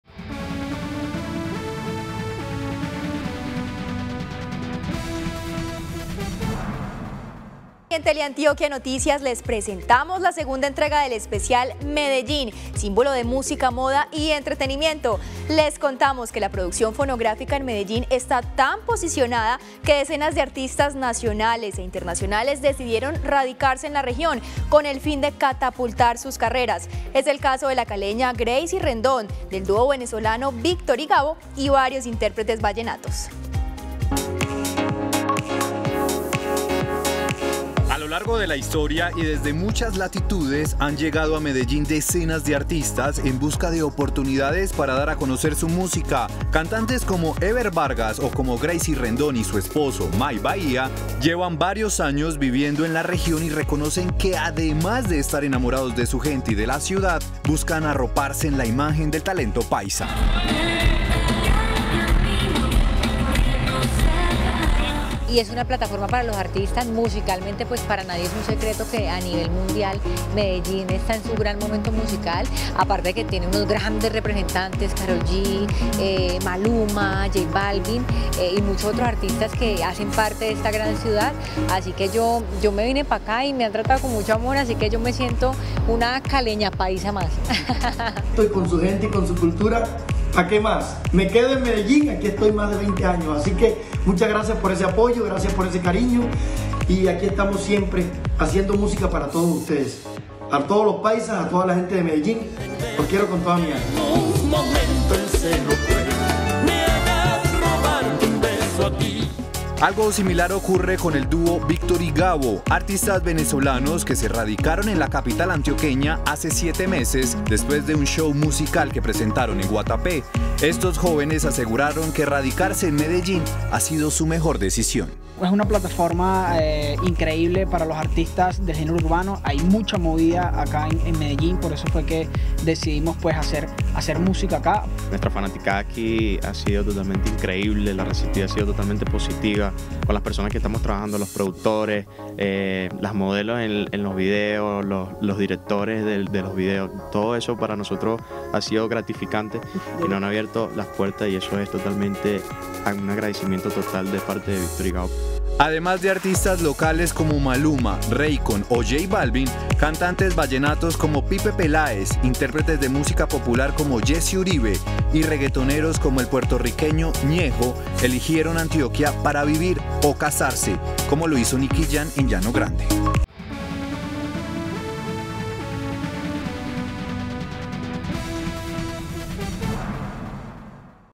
Plataforma musical de artistas de todo el mundo - Teleantioquia Noticias
La producción fonográfica en Medellín está tan posicionada que decenas de artistas nacionales e internacionales decidieron radicarse en la región, con el fin de catapultar sus carreras. Es el caso de la caleña Greeicy Rendón, del dúo venezolano Víctor & Gabo y varios intérpretes vallenatos.